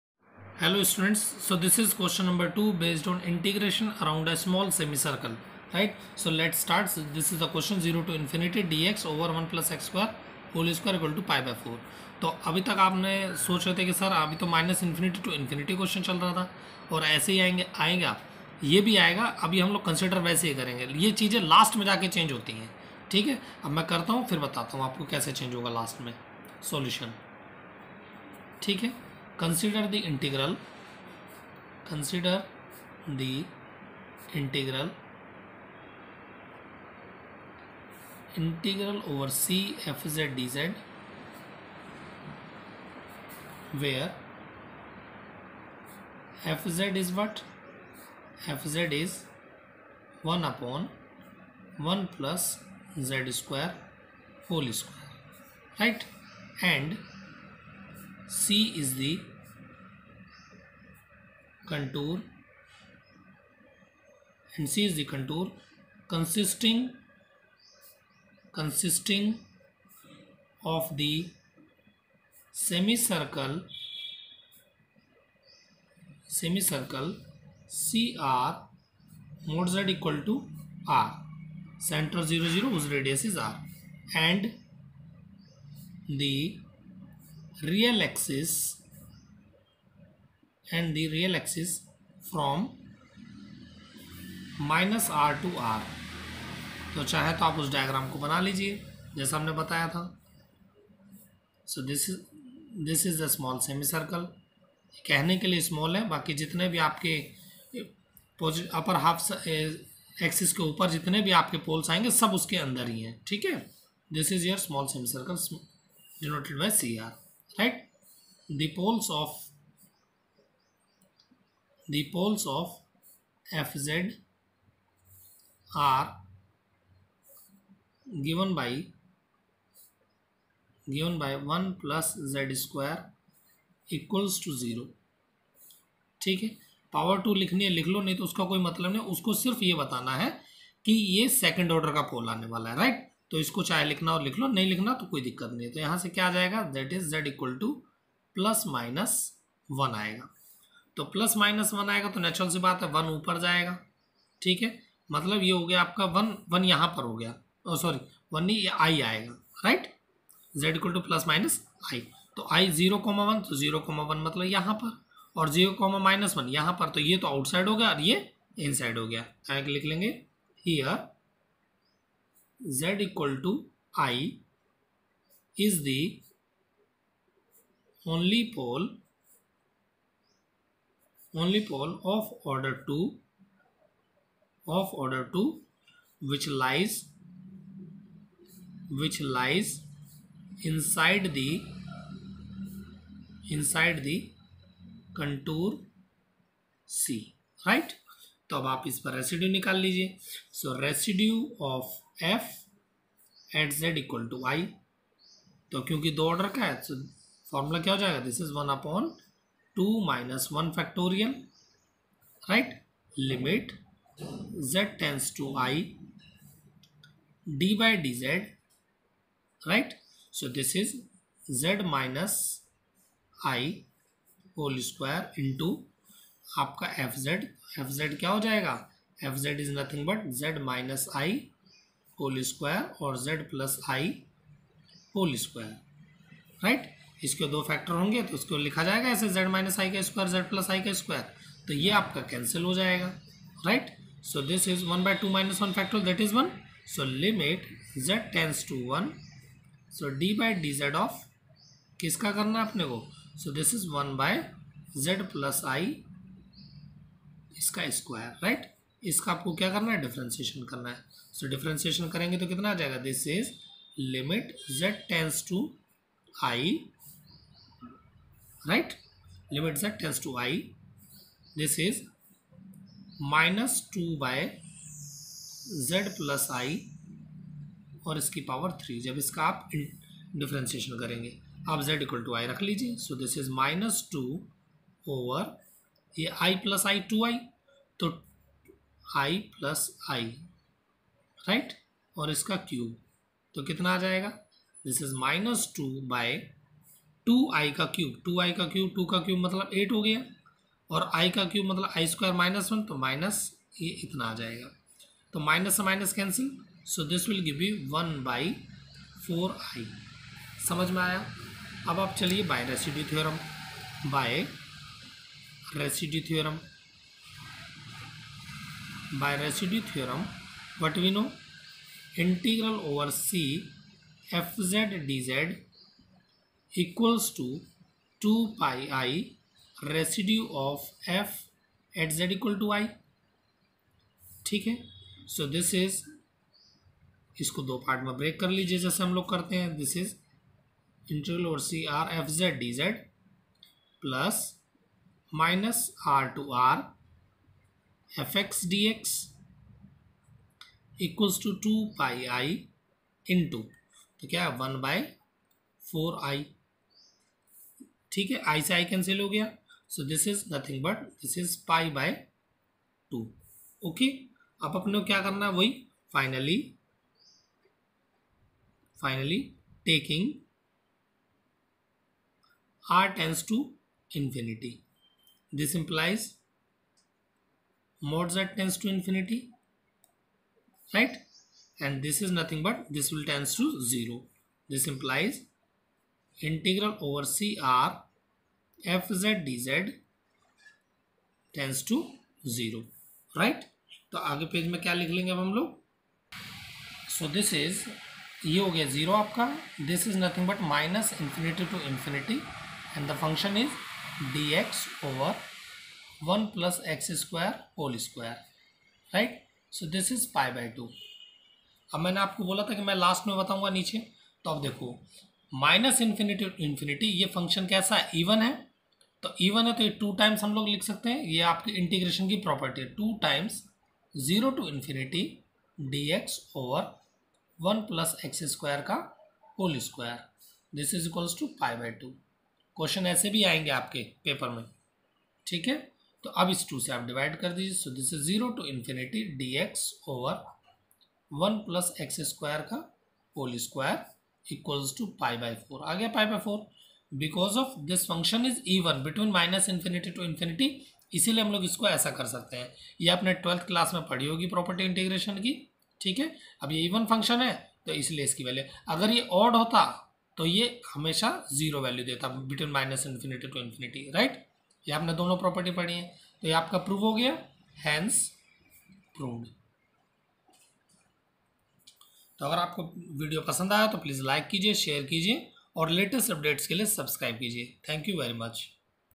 Question 2 on Integration Around A Small Semi-Circle(by Residue Method)
Evaluation of Real Definite Integral. Complete Solution to Question 2 on Integration Around a Small Semi Circle. Finding Poles or Singularities Inside the Contour or Semi Circle. Finding residues on poles which lies inside the Semi Circle. Applied Cauchy Residue theorem or Residue Theorem. SEM II Engineering Maths.